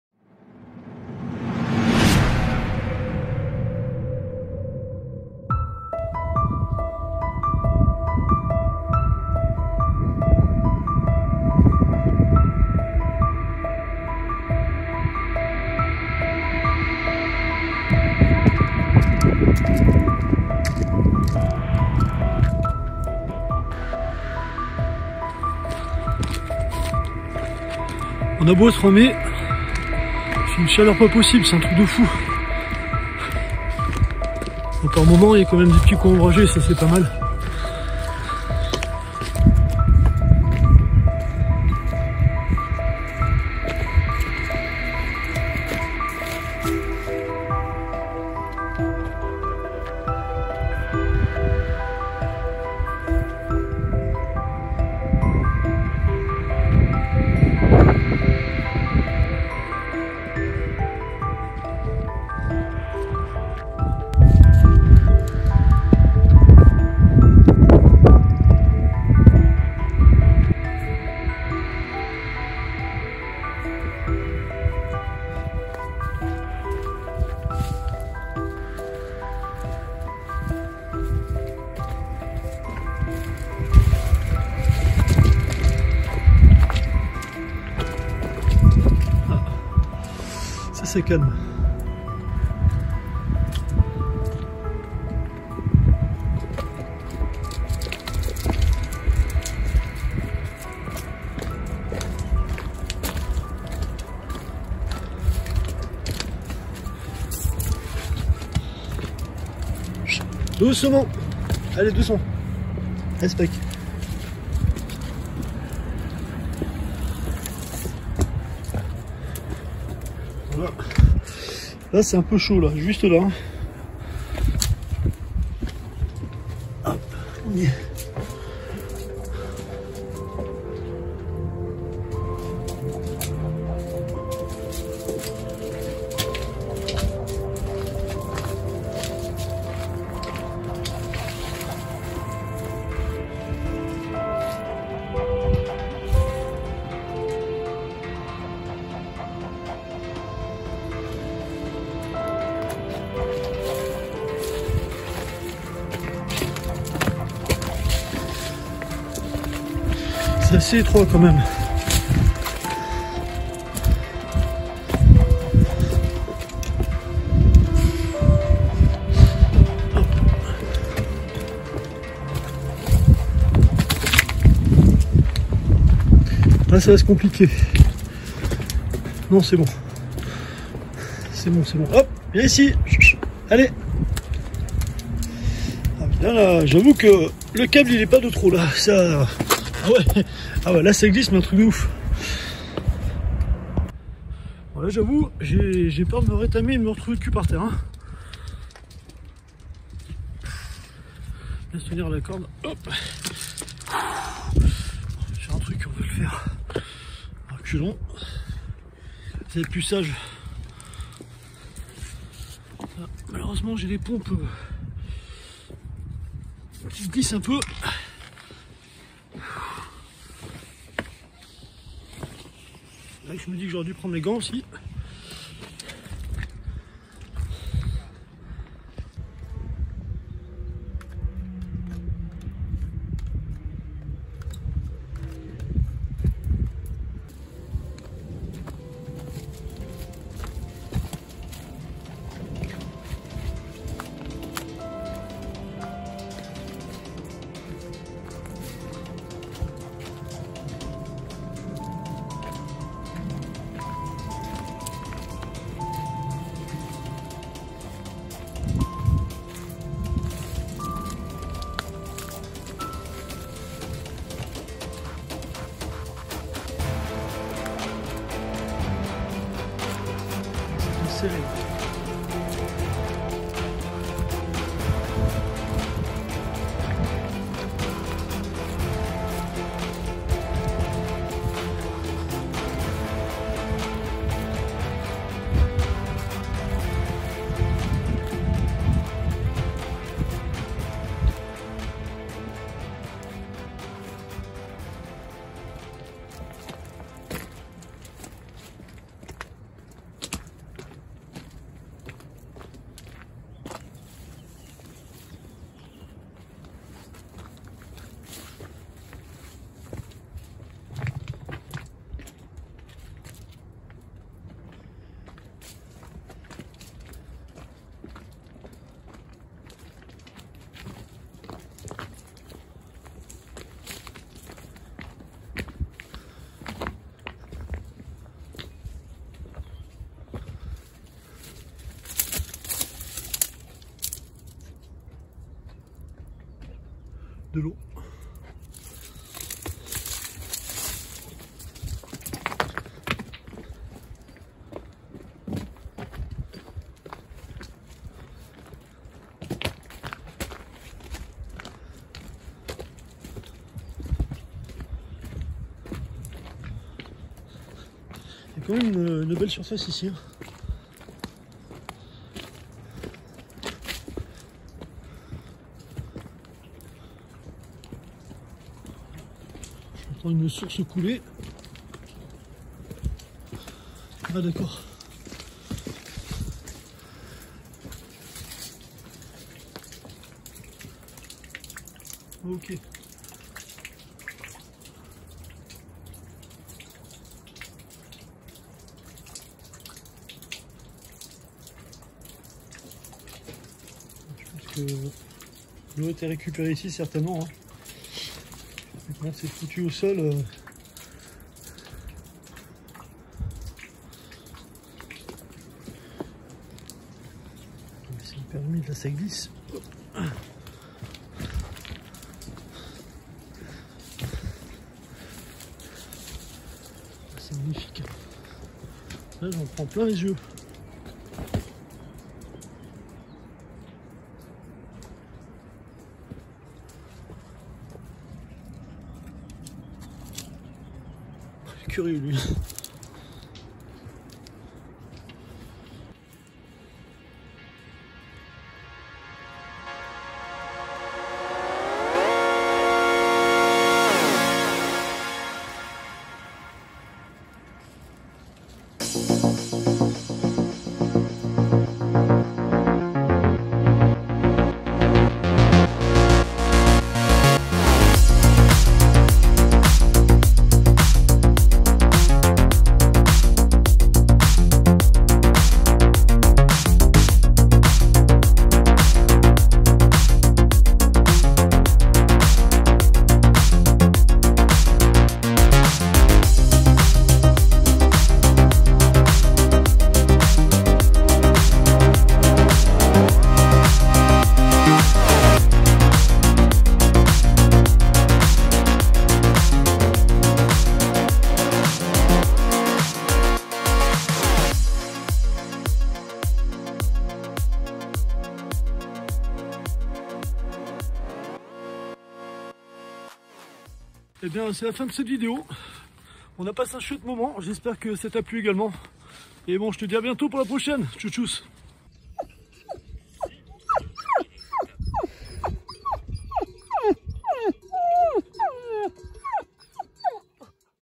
EXPLORATION PERILLEUSE : A LA DECOUVERTE D UNE FORTERESSE TROGLODYTE
Nous partons à la découverte de la forteresse troglodyte de Gourdon.
Vestiges du temps perchés en bordure de falaise avec plus de 300 mètres de vide. Son origine est controversé, personne ne sait vraiment.
Son accès est extrêmement dangereux, un passage large d'une trentaine de centimètres mène à ses portes.